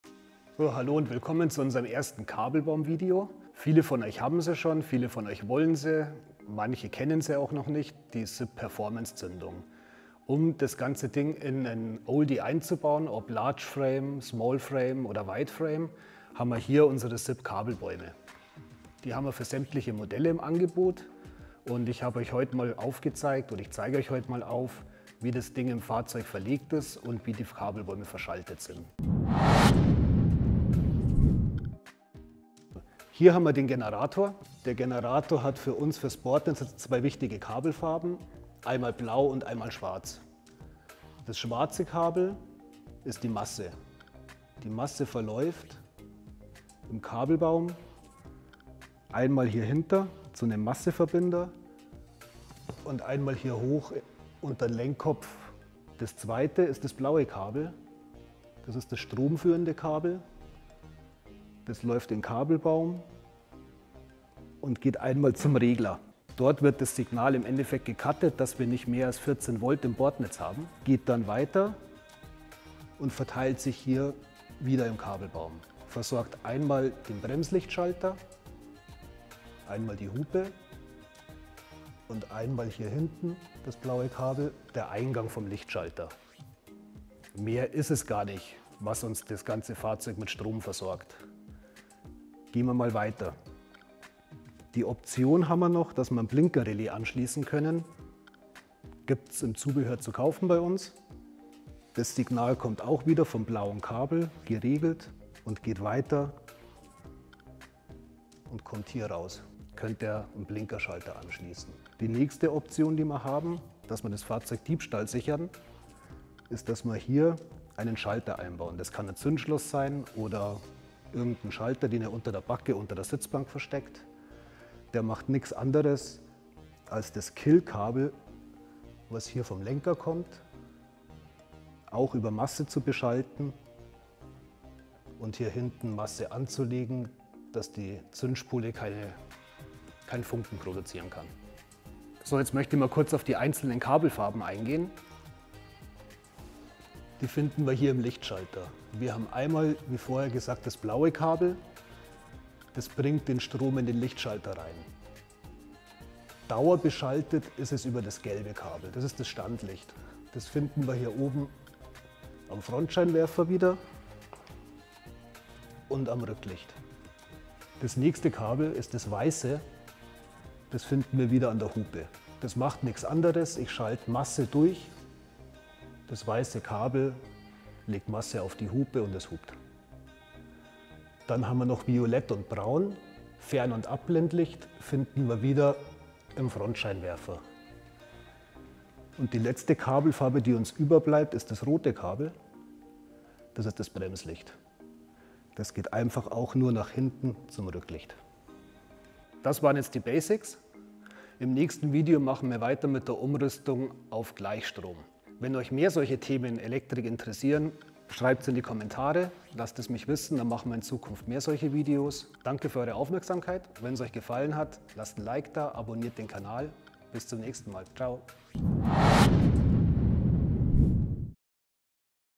Kabelbaum Basics für das richtige Tuning SIP Tutorial Folge 1 🛵🔧 {deutsch}
Du hast Dir einen der SIP Kabelbäume zur Umrüstung auf verschiedene Zündungen geholt und suchst weitere Informationen? Oder überlegst dir einen anzuschaffen bist aber noch nicht mit der Materie vertraut? Dann hat SIP ab sofort die passenden Videos für Dich!

Um Euch bei diesem Thema jetzt noch besser zu unterstützen, erklärt Euch Christoph in der „SIP Kabelbaum-Videoserie“ alles Wichtige, damit die Umrüstung auf Tuning Zündungen noch einfacher gelingt. Außerdem gibt es in jeder Folge nützliche Tipps und Tricks für die Installation mit interessantem Hintergrundwissen.

► In der ersten Folge erklärt Euch unser Elektrik-Experte Christoph die Basics der Kabelbäume, wie Ihr diese richtig verlegt und wie sie verschaltet sind.

Kapitel:
00:00 Intro
00:32 Der Aufbau
01:35 Optionaler Anschluss eines Blinkerrelais
01:52 Optionale Diebstahlsicherung
02:21 Die Kabelfarben
03:19 Outro

Das Zubehör
✅ Kabelbaum SIP für Vespa 50 N/ L/ R/ S/ SR/ SS/ 90/ R/ SS/ 100/ 125 PV/ ET3

✅ Kabelbaum SIP für Vespa 125 VNA-TS/ 150 VBA-T4/ Rally

Hat dir das Video gefallen? Schau mal in unserem Shop vorbei Hast du Fragen? Dann schreib uns in den Kommentaren.